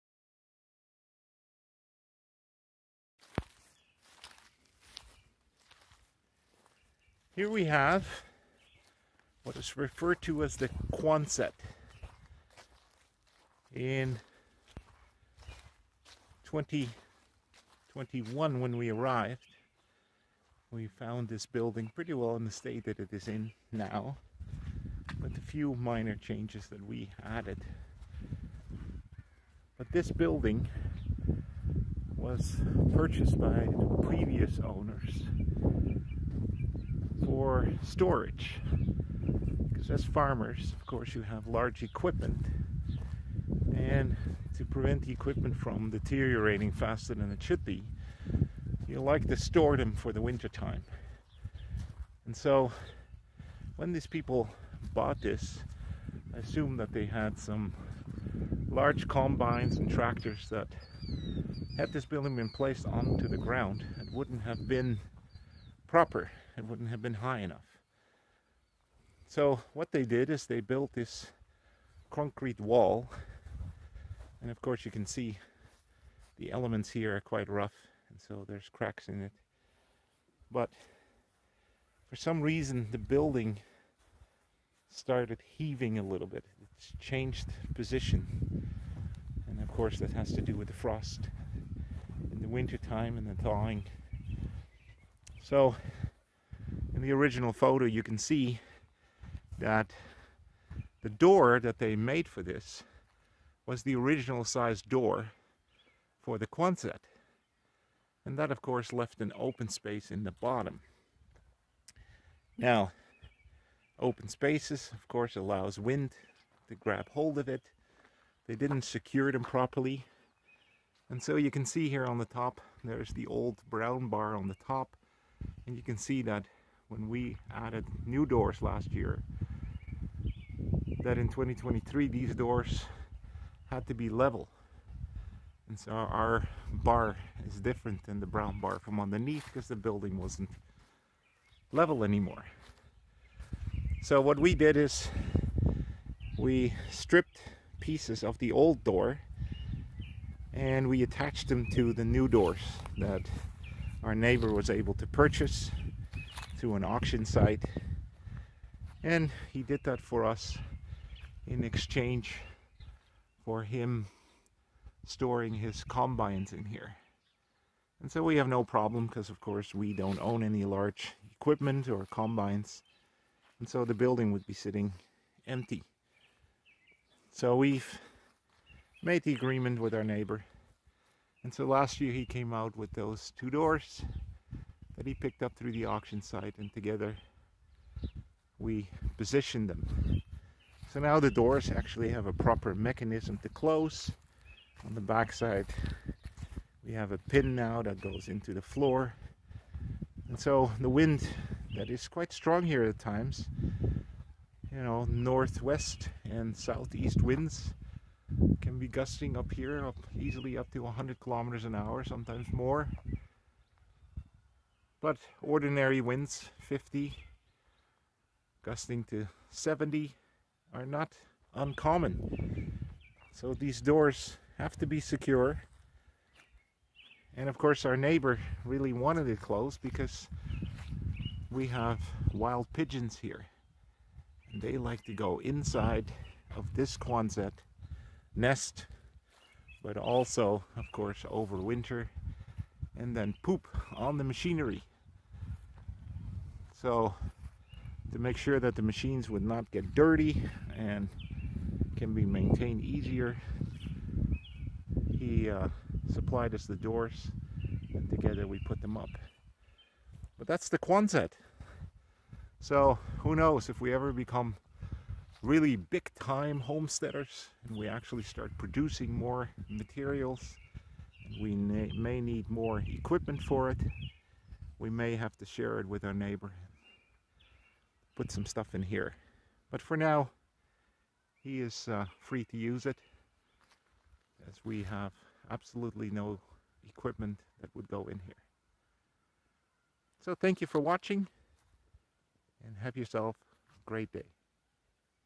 Quonset homestead family farm rural saskatchewan building
Here a short video about one of our outbuildings and what we have been able to do to it to improve it.